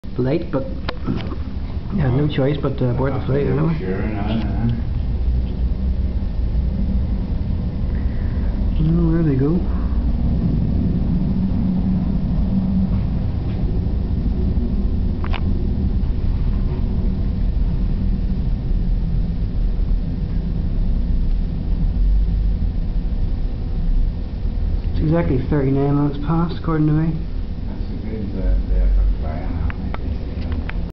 Easyjet Belfast - Berlin takeoff 18/07/06
Boeing 737 departs Belfast International for Berlin.